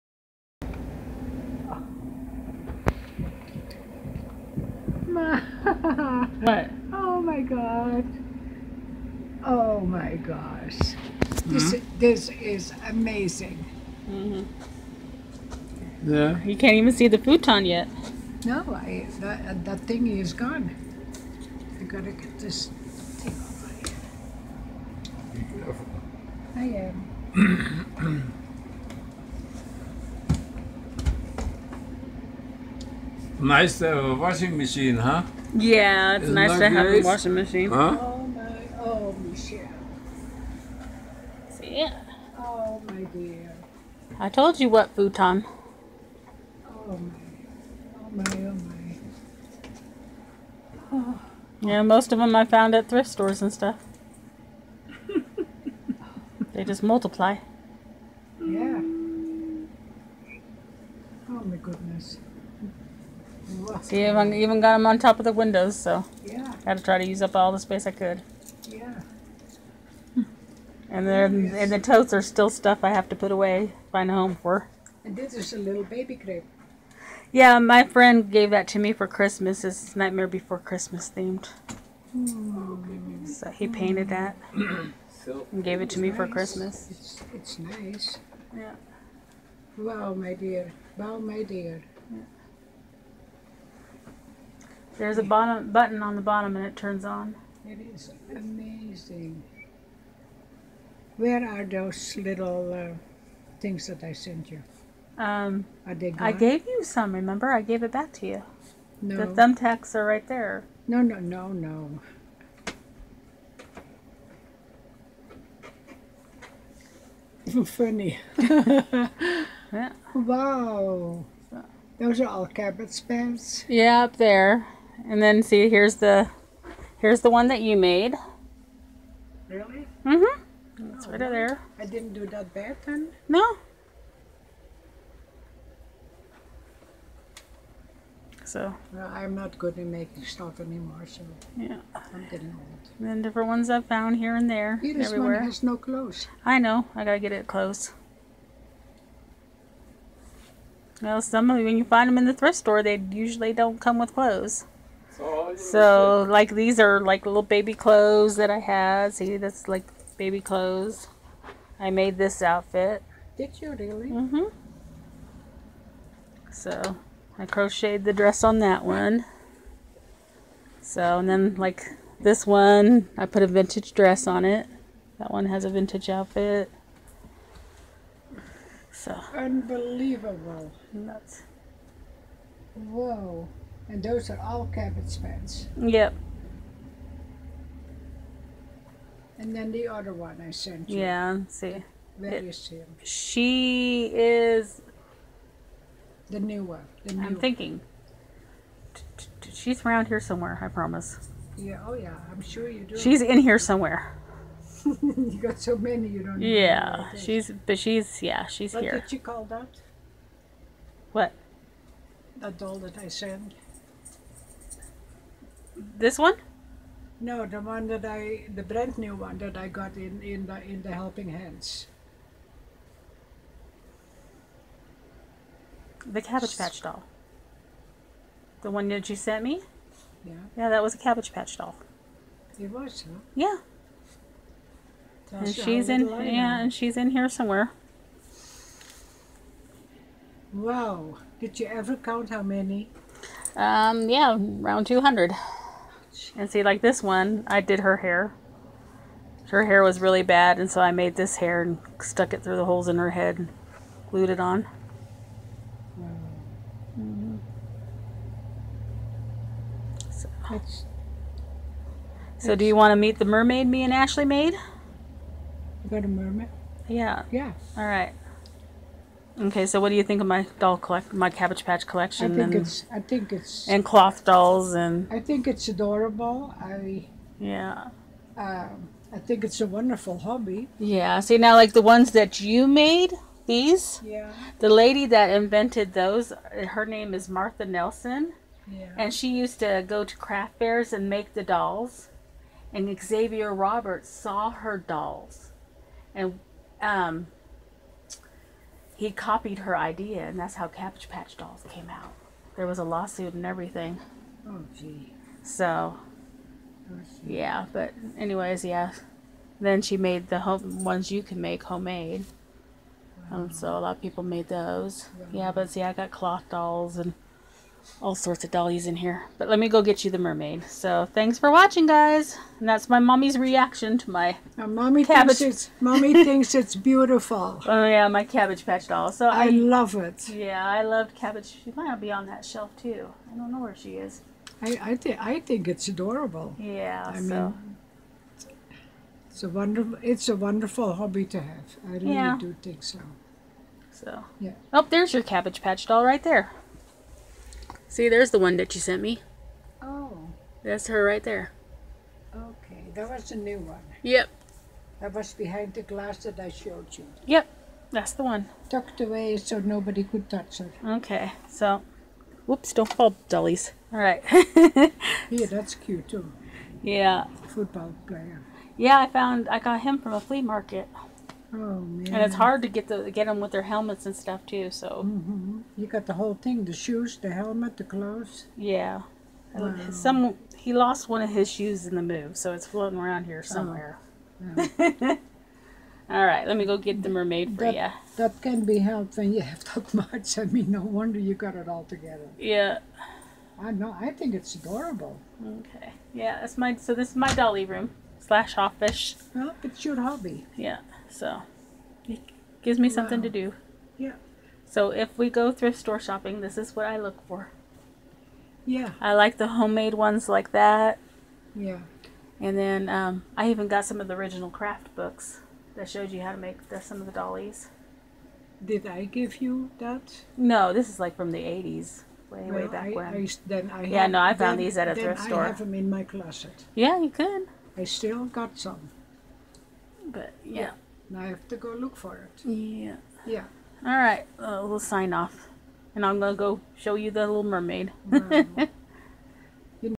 My Mom's reaction to my Cabbage Patch doll room collection
My Mom's reaction to my cabbage patch doll room.  Thank you for eatching!